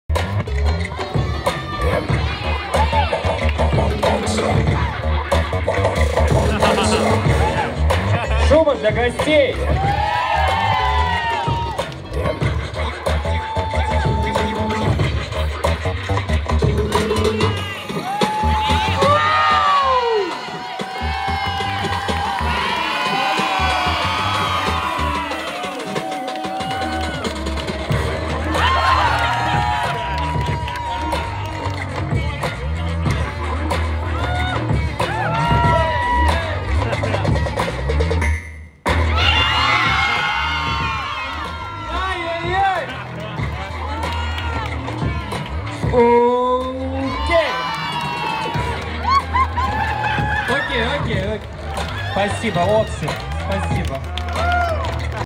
Оксид / 2 круг / The last hero
The last hero battle
07/08/21
Under Summer Camp

Танцевальная студия Under Stand
Воронеж, Студенческая,17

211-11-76